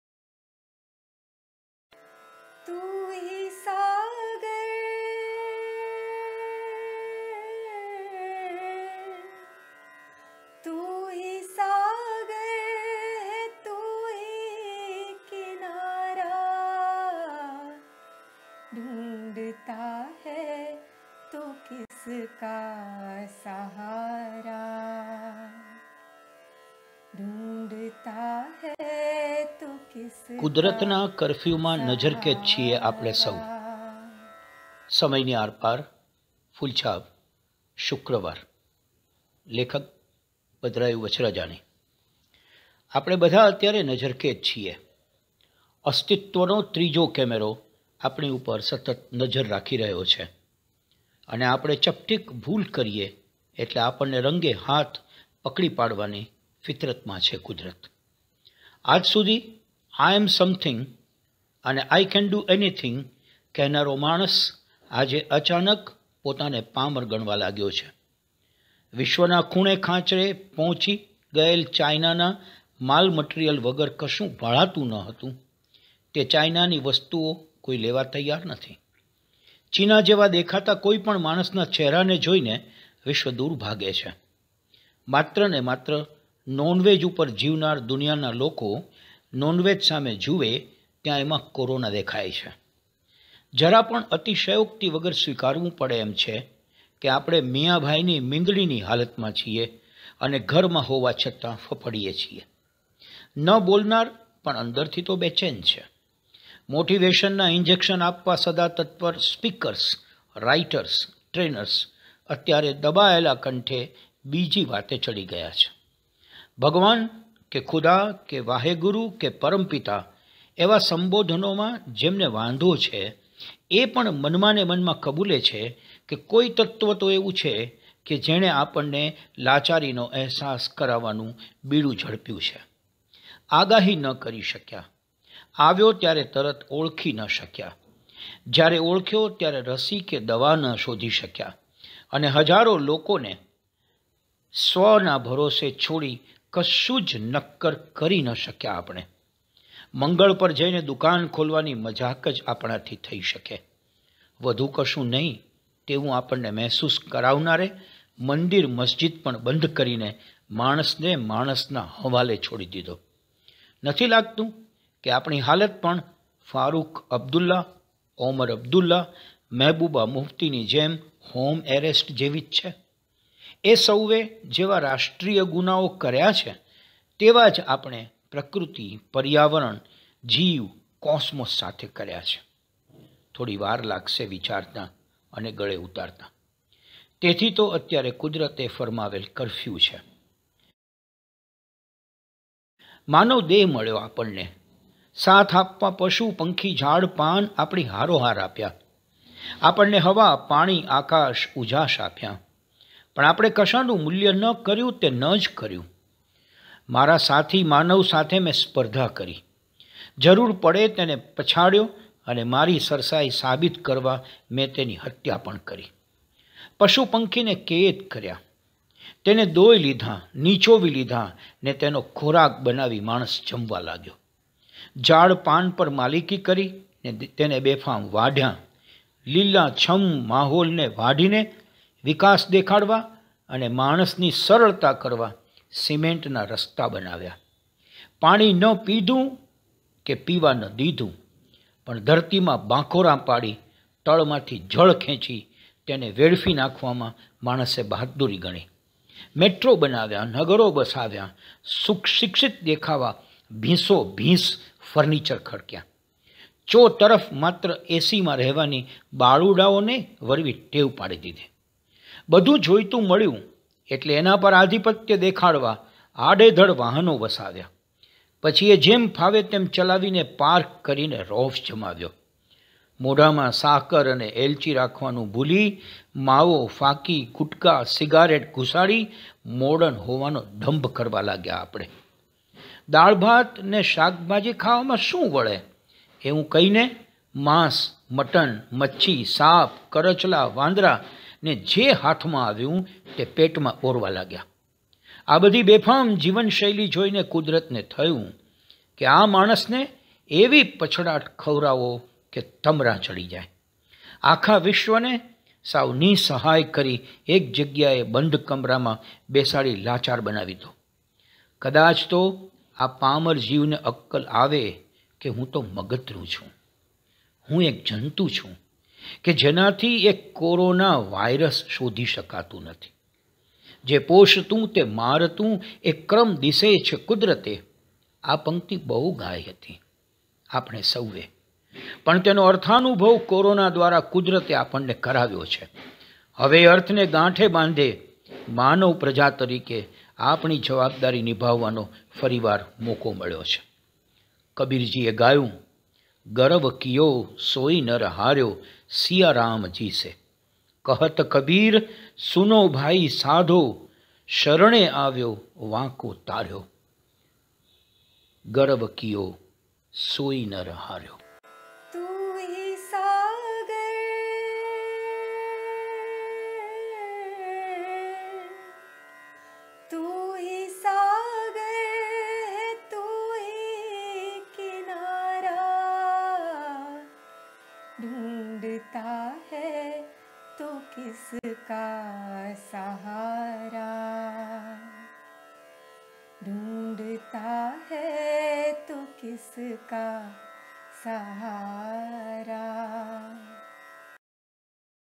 || Bhadrayu :: કુદરતનો કફર્યુ ||
અસ્તિત્વનો ત્રીજો કેમેરો સતત આપણી ઉપર મંડાયો છે અને આપણાં કર્યાં આપણે ભોગવવાનો વારો આવ્યો છે !
કબીરજી સાચું કહેતા હતા: ગરવ કિયો સોઈ નર હાર્યો !!
માત્ર પાંચ મિનિટ ખુદ માટે

Bhadrayu Vachhrajani